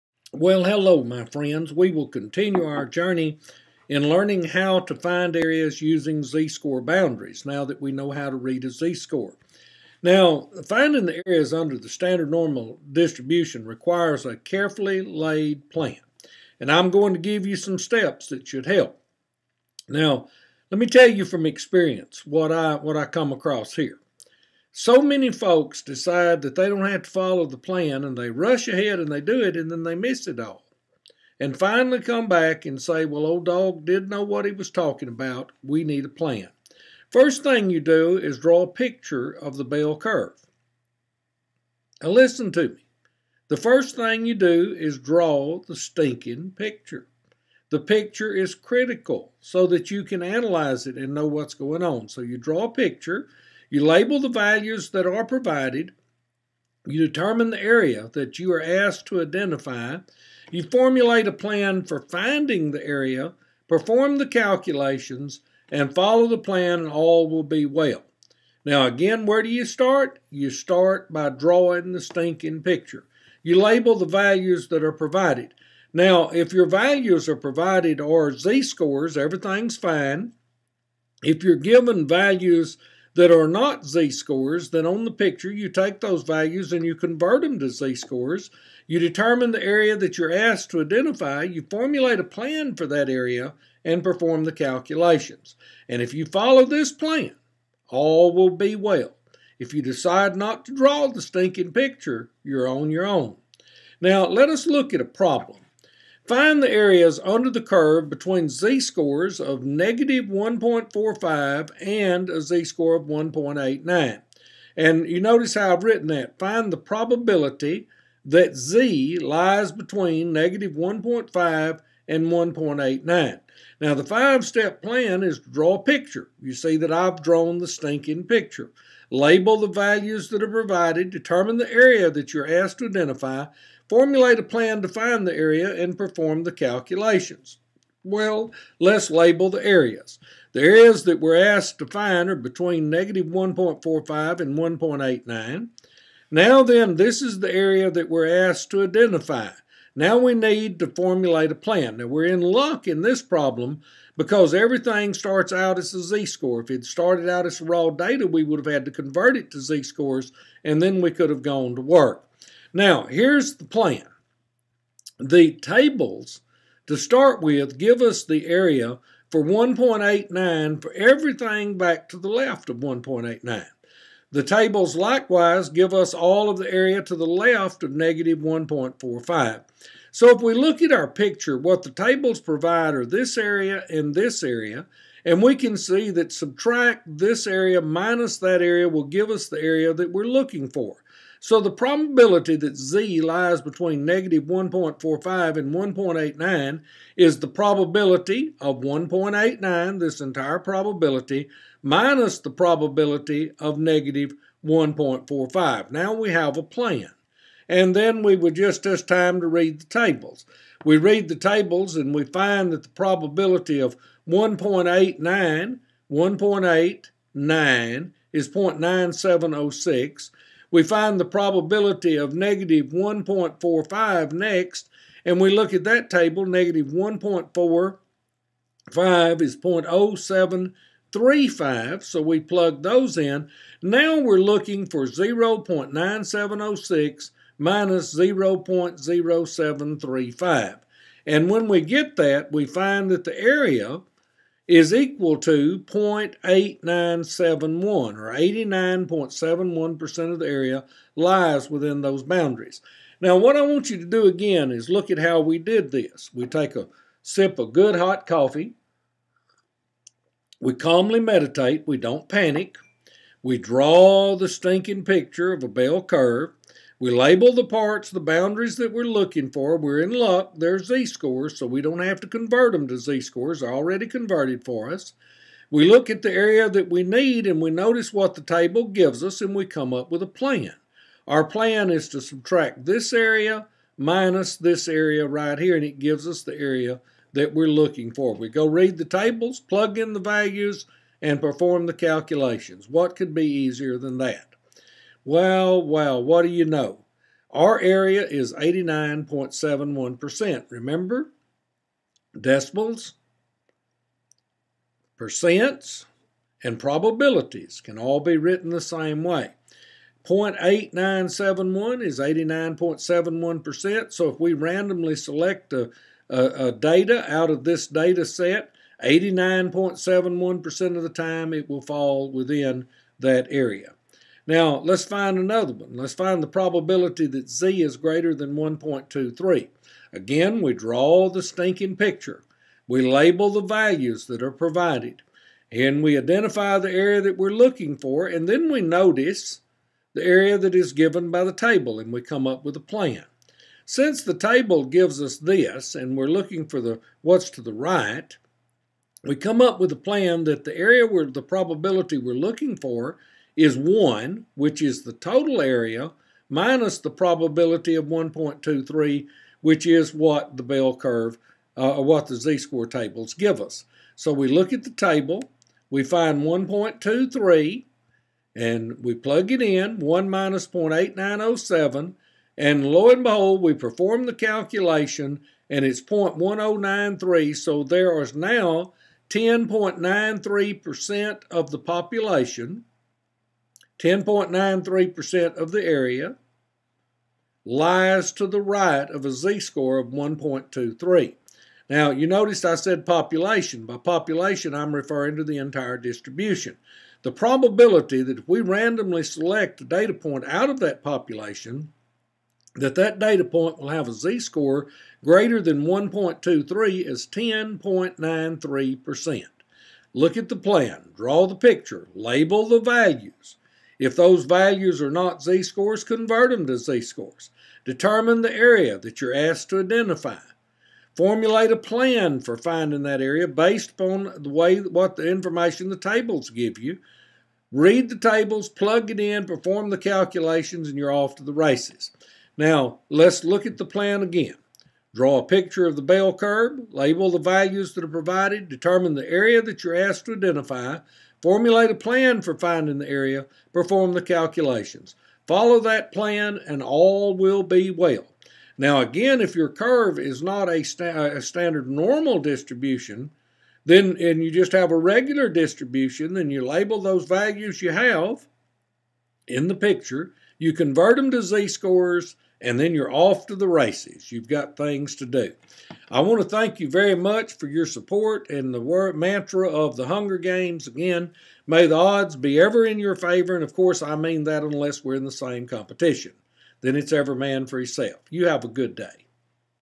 5 Module 2 Plan for Finding Areas
This video provides a plan for finding areas under the normal distribution curve.